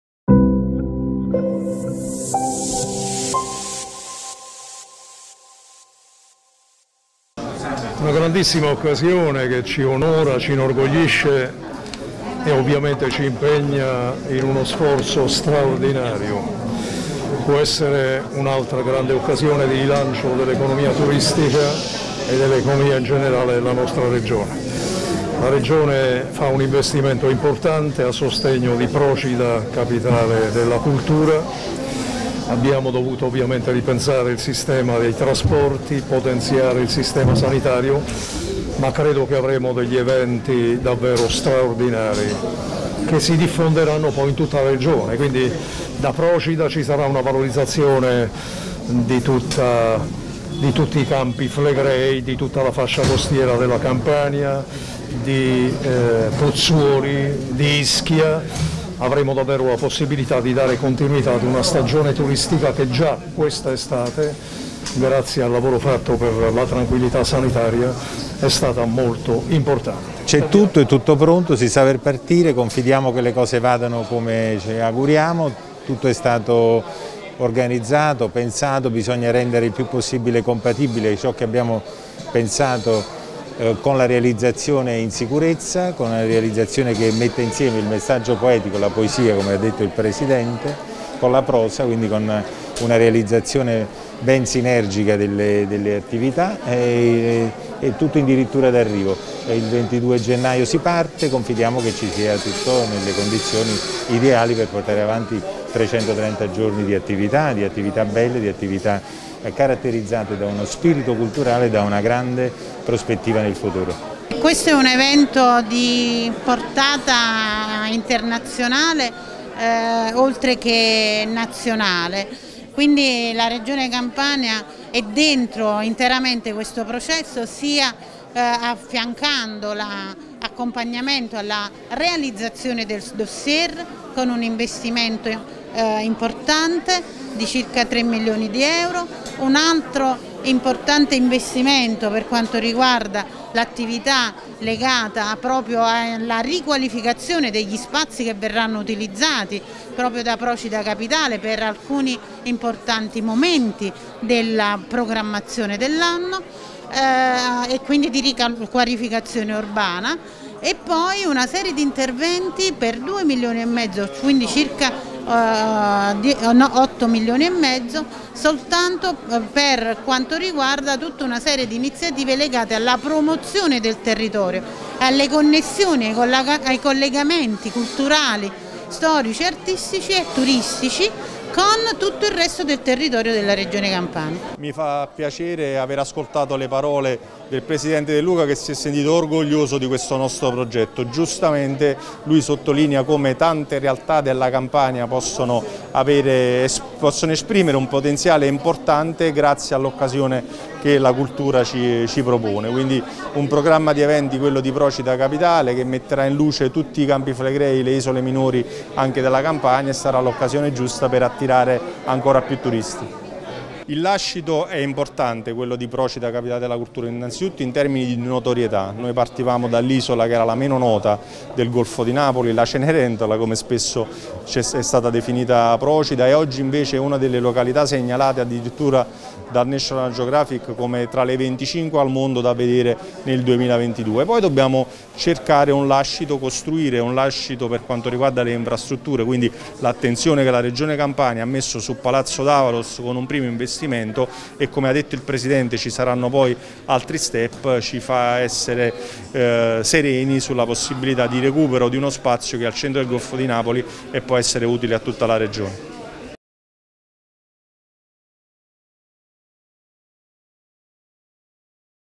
Il programma di Procida Capitale: la cultura non isola
150 eventi distribuiti in 330 giorni di programmazione: l’anno della nuova Capitale Italiana della Cultura inizia il 22 gennaio. Coinvolti 350 artisti di 45 Paesi differenti. Dimensione internazionale, co-creazione, inclusione ed ecosostenibilità le parole-chiave.

44 progetti culturali (di cui 34 originali), 150 eventi distribuiti in un cartellone di 330 giorni di programmazione, 350 artisti provenienti da 45 Paesi differenti del mondo e il coinvolgimento diretto di oltre 2000 cittadini, con la rigenerazione di 7 luoghi simbolo dell’isola.